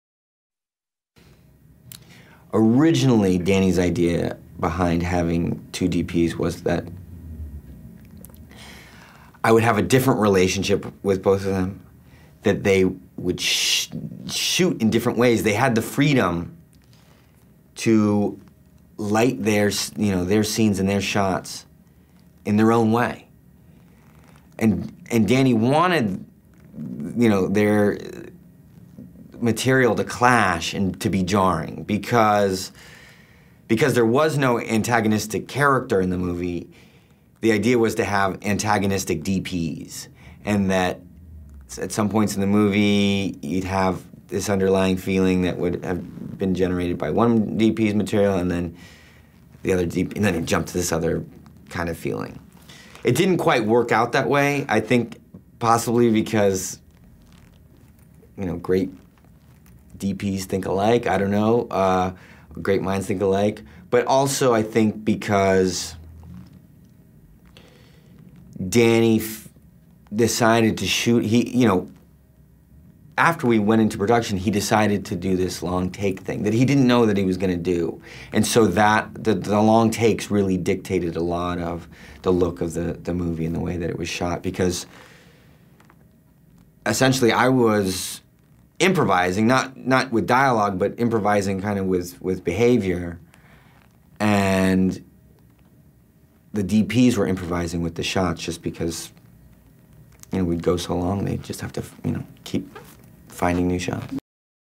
James Franco - '127 Hours' [HD]
Interview - James Franco "On Working with two DPs".
Toronto International Film Festival 2010.